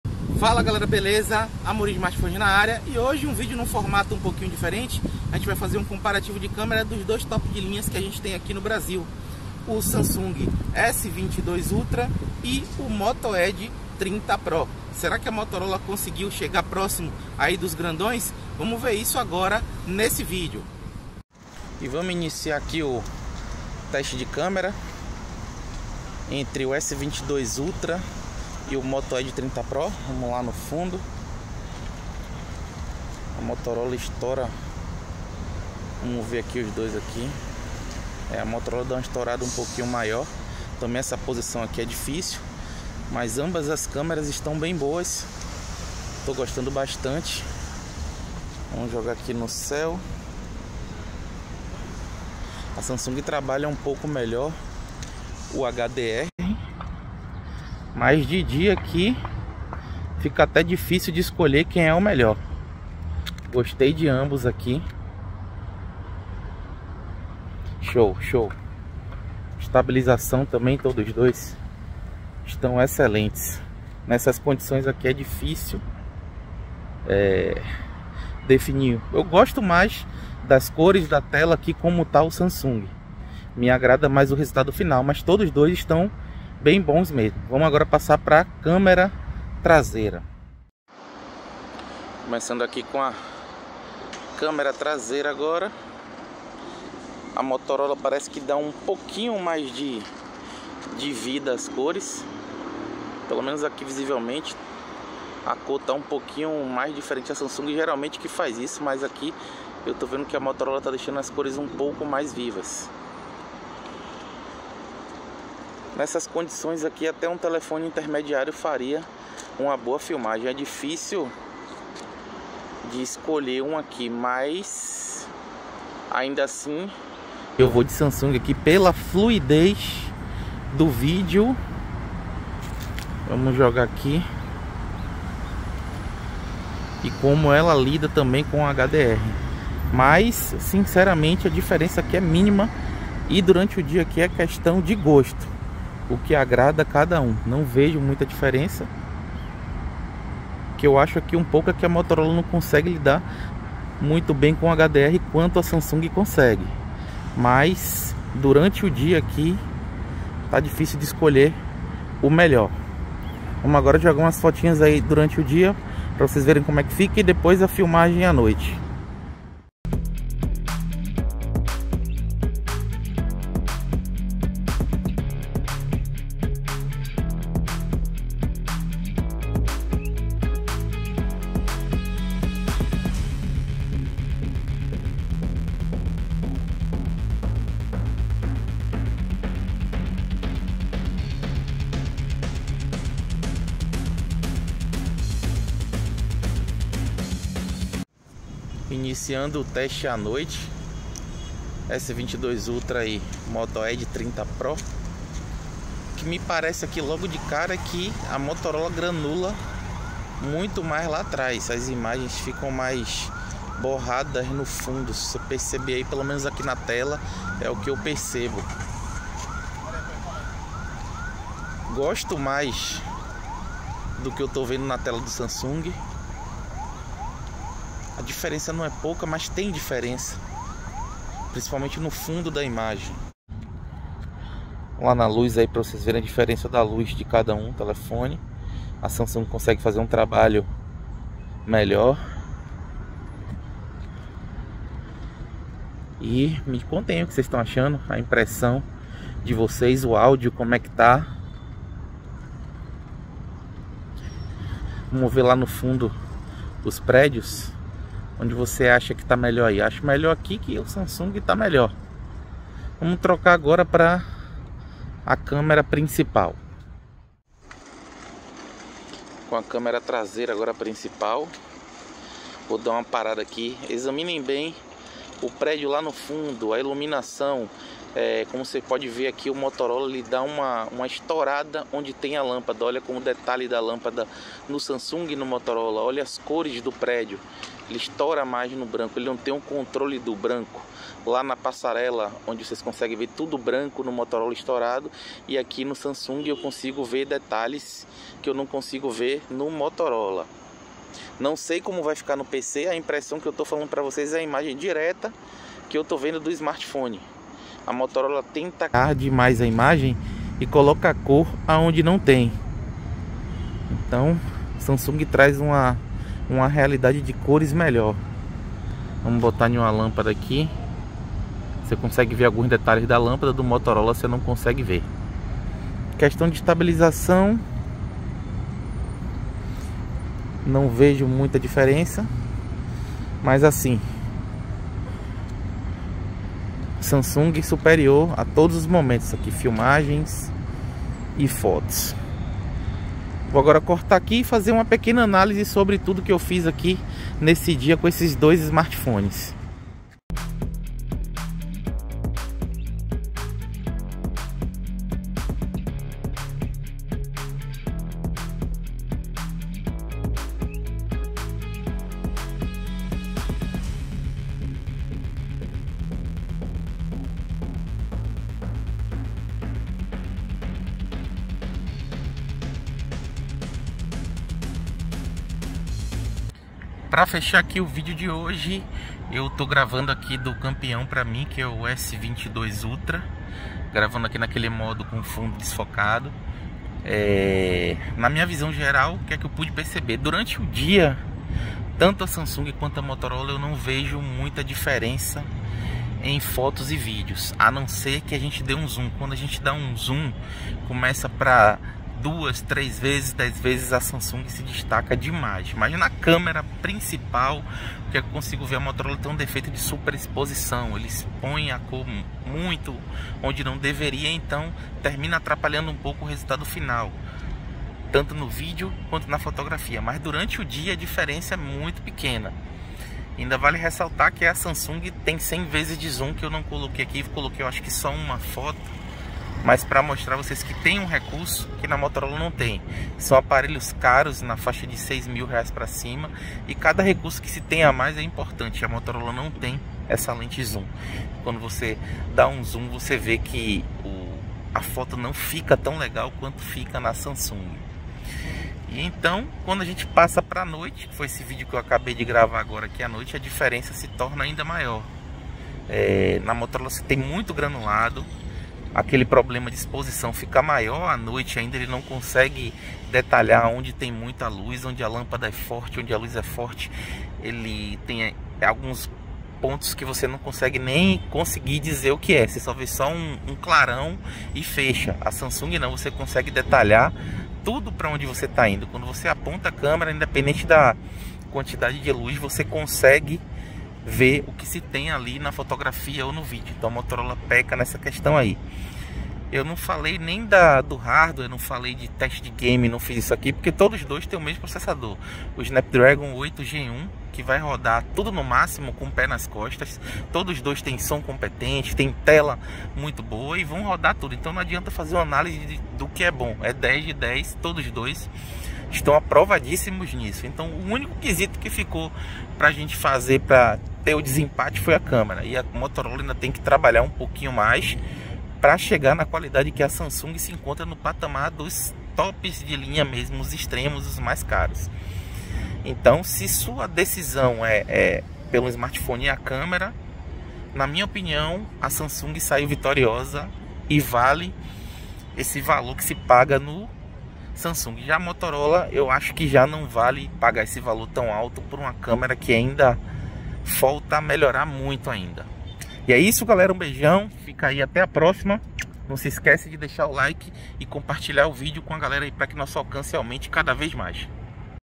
Samsung S22 Ultra Vs Moto Edge 30 Pro
Vídeo gravado com Samsung S22 Ultra e Moto Edge 30 Pro
Edição Feita no aplicativo Movavi para PC